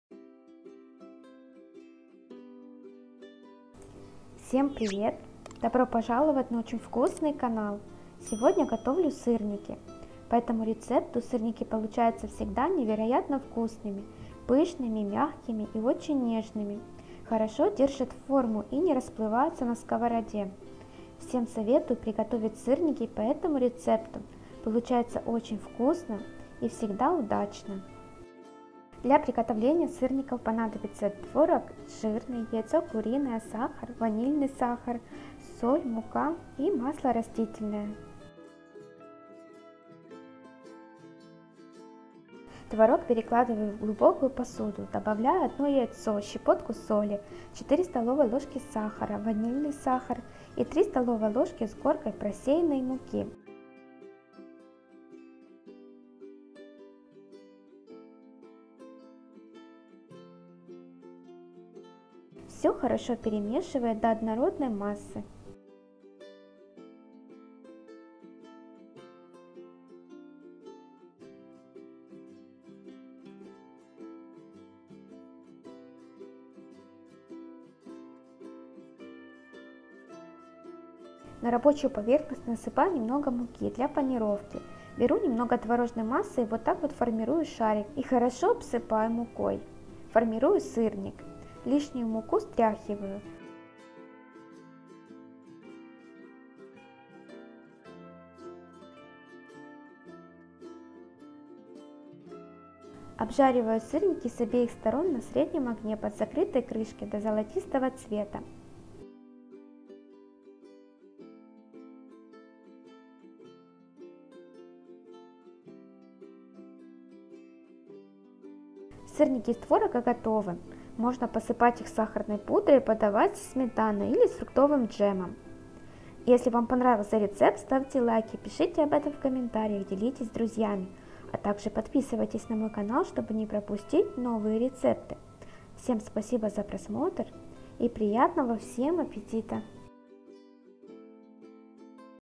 Нежные Сырники Из Творога. Лучший Рецепт.
Привет друзья! Вы зашли на очень вкусный канал! Сегодня готовлю сырники. По этому рецепту сырники получаются всегда невероятно вкусными, пышными, мягкими и очень нежными, хорошо держат форму и не расплываются на сковороде. Всем советую приготовить сырники по этому рецепту, получается очень вкусно и всегда удачно. Мне очень важно ваше мнение оставляйте под видео свои комментарии, отзывы и пожелания, а также делитесь своими любимыми рецептами. Приятного аппетита!!)).

Необходимые ингредиенты:
Товорог жирный - 400-500 гр.
Ванильный сахар - 1 пакетик
Соль - щепотка
Мука - 3 ст. л. (и приблизительно столько же для панировки)
Масло растительное (для жарки) - 4 ст. л.

Для поддержки нашего канала Очень вкусный канал:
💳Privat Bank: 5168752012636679